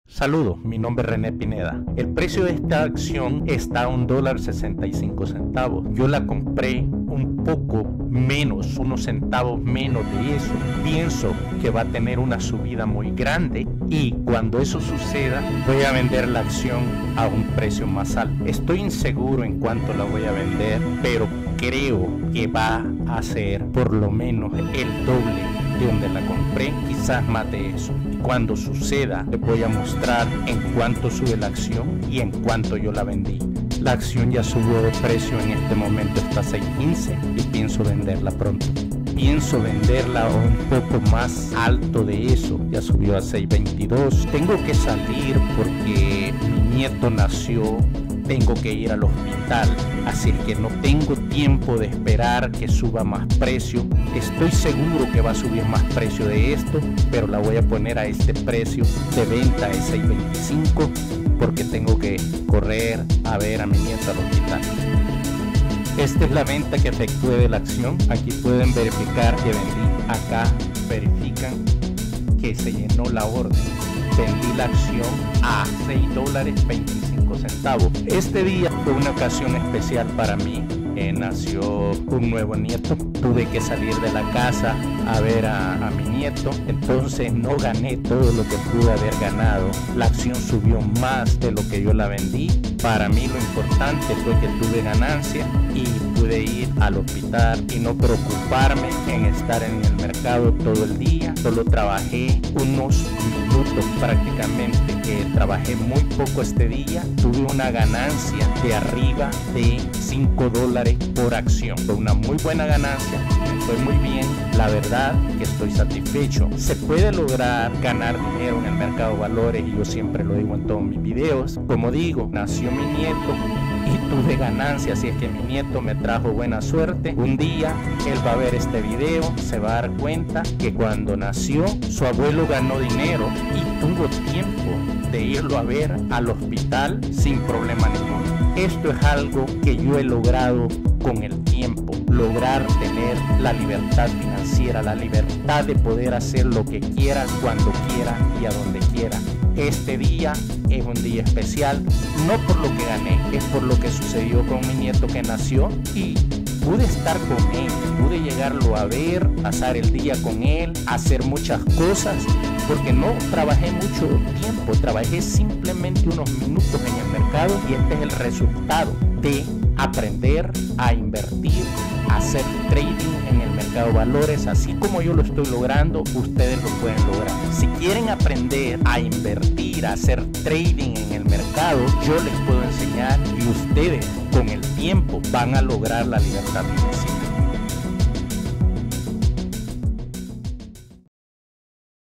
NACIMIENTO DE BEBE Y GANANCIA EN EL MERCADO DE VALORES
Compre las acciones de esta empresa en $1.63 por accion  y la vendi en $.6.25. por accion .Esto sucedio el dia que nacio mi nieto y no gane mas por accion, por que fui al hospital a ver al nuevo bebe. No pedi permiso en mi trabajo para tener tiempo libre ya que tengo libertad de tiempo.